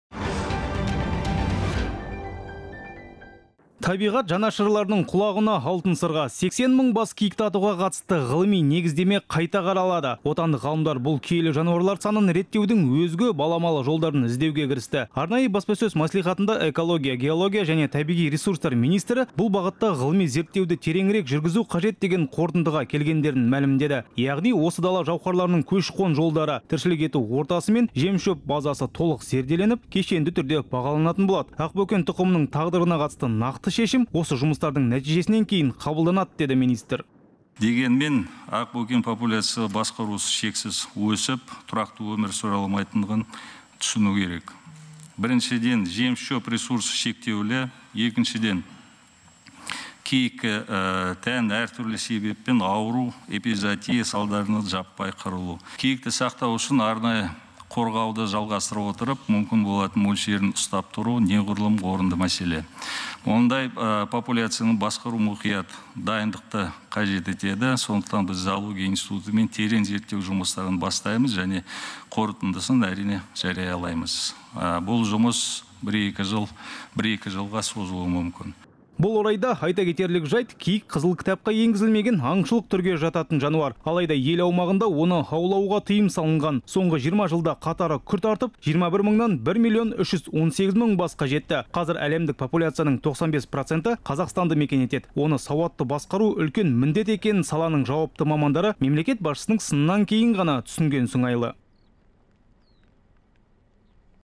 Киік санын реттеудің жаңа тәсілдері қарастырылады. Qazaq TV
Табиғат жанашырларының құлағына алтын сырға 80 мың бас киікті атуға қатысты ғылыми негіздеме қайта қаралады. Отандық ғалымдар бұл киелі жануарлар санын  реттеудің өзге баламалы жолдарын іздеуге кірісті. Арнайы баспасөз мәслихатында экология, геология және табиғи ресурстар министрі бұл бағытта ғылыми зерттеуді тереңірек жүргізу қажет деген қорытындыға келгендерін мәлімдеді. Яғни, осы дала жауһарларының  көші-қон жолдары, тіршілік ету ортасы мен жем-шөп базасы толық зерделеніп, кешенді түрде бағаланатын болады. Ақбөкен тұқымының тағдырына қатысты нақты шешім осы жұмыстардың нәтижесінен кейін қабылданады деді, министр.
Серікқали Брекешев, ҚР экология, геология және табиғи ресурстар министрі:
– Дегенмен, ақбөкен популяциясы басқарусыз шексіз өсіп, тұрақты өмір сүре алмайтындығын түсіну керек. Біріншіден, жем-шөп ресурсы шектеулі, екіншіден, киікке тән әр түрлі   ауру, эпизоотия салдарынан жаппай қырылу. Киікті сақтау үшін - арнайы қорғауды жалғастыра отырып, мүмкін болатын мөлшерін ұстап тұру неғұрлым орынды мәселе. Мұндай популяцияны басқару мұқият дайындықты қажет етеді. Сондықтан, біз Зоология институтымен терең зерттеу жұмыстарын бастаймыз және қорытындысын, әрине, жариялаймыз. Бұл жұмыс – бір-екі  жылға созылуы мүмкін.
Бұл орайда айта кетерлік жайт, киік-Қызыл кітапқа енгізілмеген аңшылық түрге жататын жануар. Алайда ел аумағында оны аулуаға  тыйым салын. Соңғы 20 жылда  қатары күрт артып 21 мыңнан 1 миллион 318 мың басқа жетті. Қазір әлемдік популяциясының 95% Қазақстанды мекен етеді. Оны сауатты басқару үлкен міндет екенін саланың жауапты мамандары мемлекет басшысының сынынан кейін ғана түсінген сыңайлы.